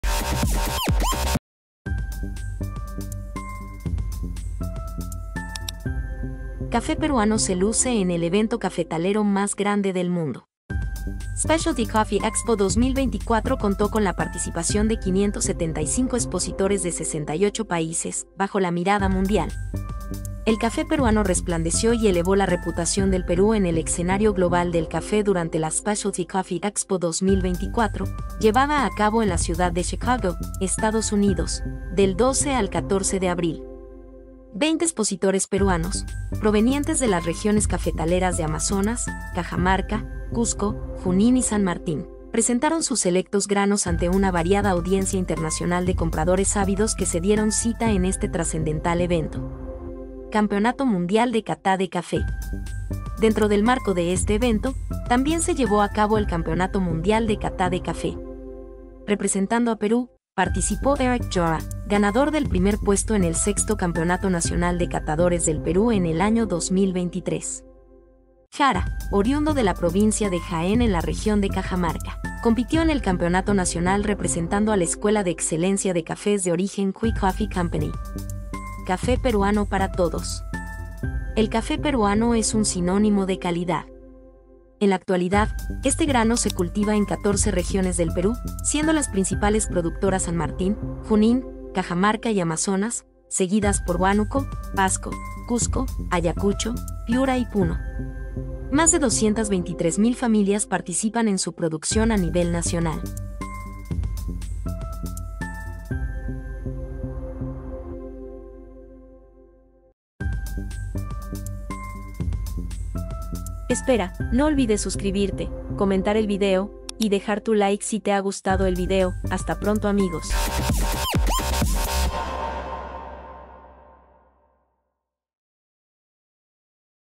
¡NOTICIA HOY! Cafè Peruano se Luce en expo de EEUU chicago 2024
Cafè Peruano se Luce en expo de EEUU chicago 2024

✨ ¡El café peruano brilló en la Specialty Coffee Expo 2024 en Chicago! ☕️🌟 Veinte expositores de regiones como Amazonas, Cajamarca, Cusco, Junín y San Martín cautivaron a compradores internacionales con sus granos de especialidad. Además, ¡Erick Jara representó a Perú en el Campeonato Mundial de Cata de Café! 🏆🌎 El café peruano es calidad garantizada, producido por más de 223,000 familias en todo el país. 🇵🇪💼